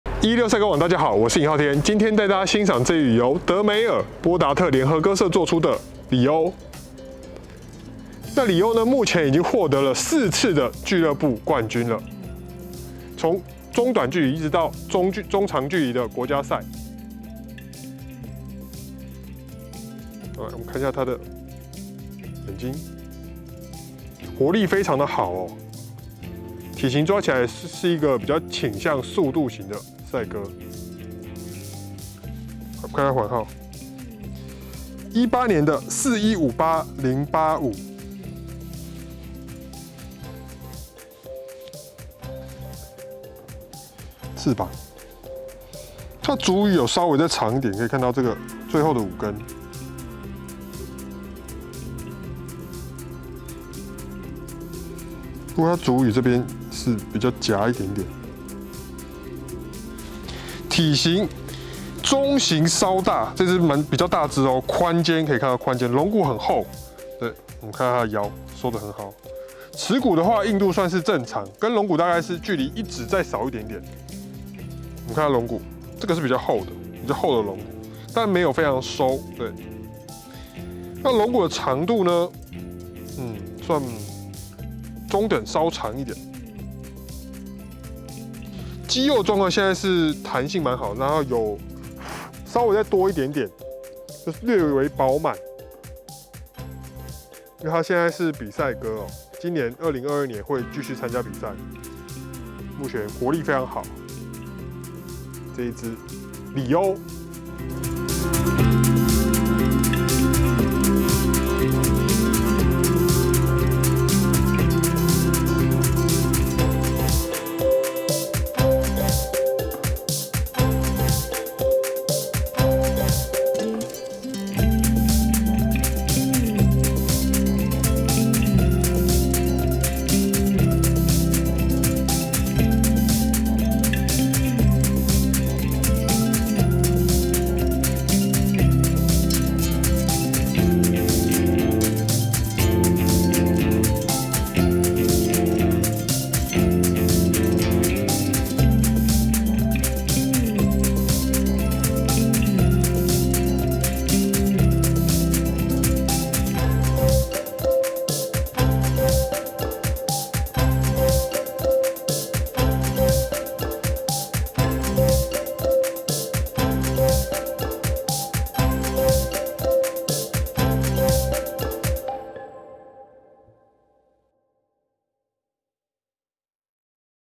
【名鴿欣賞】2020年查特盧4800羽省賽2位丨里歐 B18-4158085丨德梅爾-波達特
@名鴿欣賞 @德梅爾-波達特 @維姆.波達特 @De Meyer-Boddaert @Boddaert wim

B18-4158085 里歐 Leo
作出者：德梅爾-波達特 De Meyer-Boddaert
本身成績：
20-查特盧4800羽省賽2位
20-亞精頓3695羽省賽11位
20-波　治4858羽省賽209位
20-威爾森149羽1位
19-圖　瑞231羽1位

拍攝日期：2022年4月